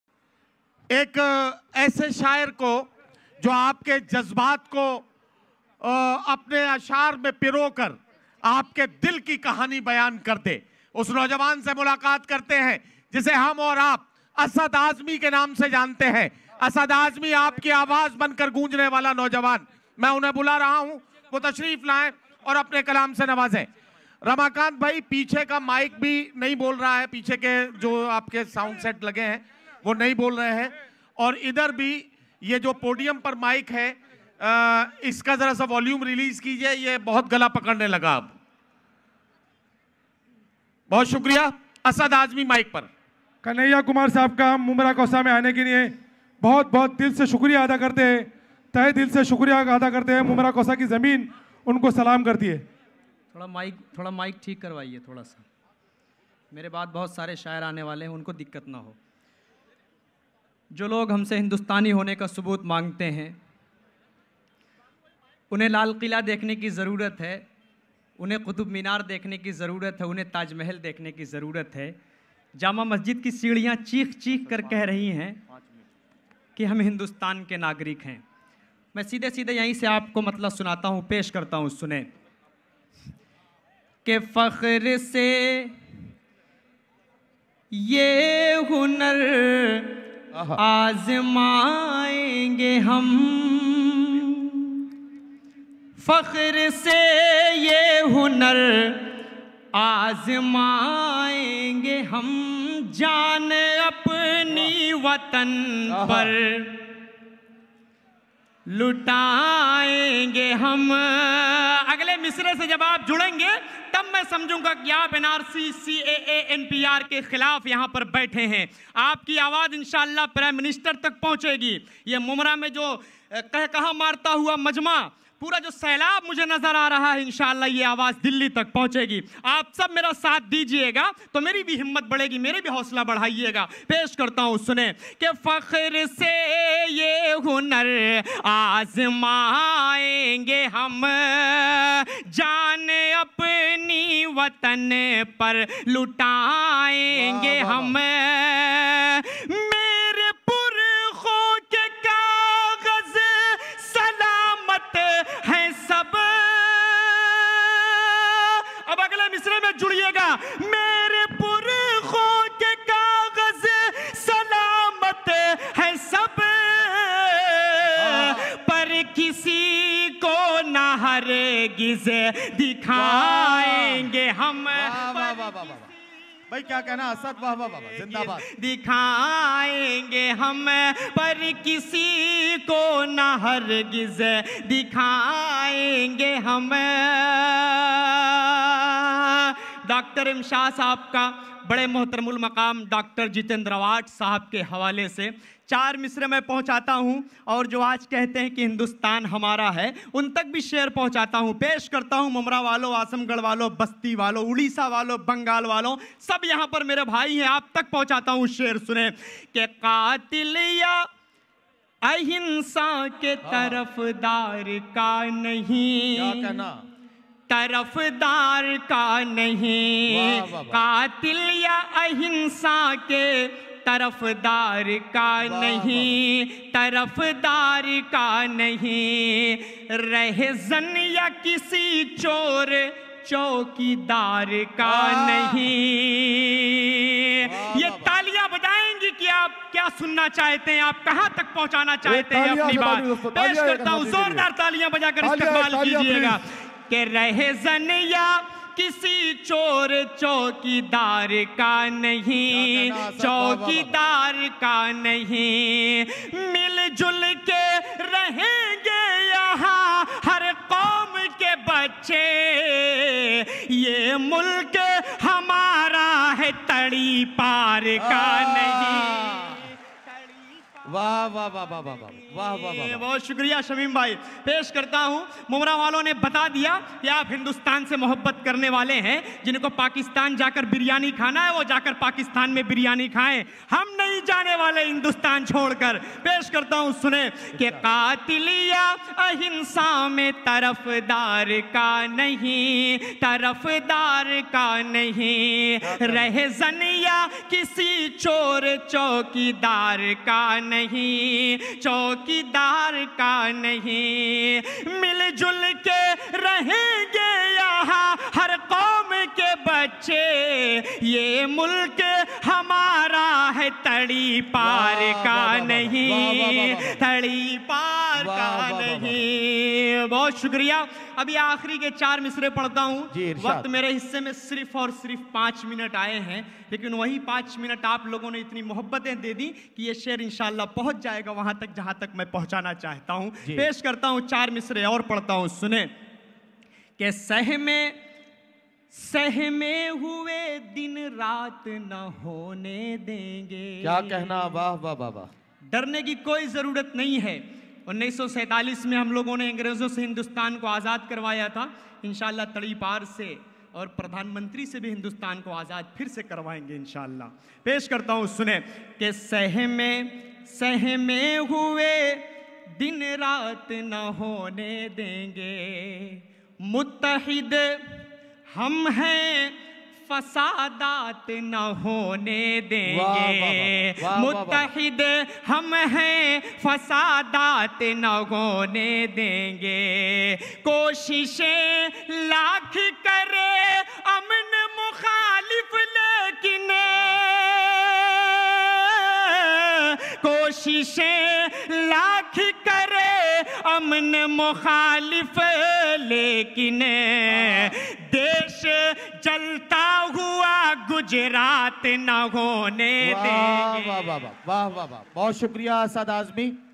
असद आज़मी शायर ने अमित शाह मोदी को ललकारा | Ahtejaji Mushaira 2020 ||asad azmi shayar | Prahar Times
Mumbra Me Ahtejaji Mushaira 2020 |asad azmi shayar | NRC, NPR, CAA, ACT KE KHILAF | Prahar Times
Mumbra Me Ahtejaji Mushaira 2020 | ASAD AAZMI | Prahar Times
ncr npr caa kanoon ke khilaf mudahira mumbra ke mittal graund me rakha gaya tha jaha par shayaron ne delhi ke takht se lekar amit shah modi ko nishana sadha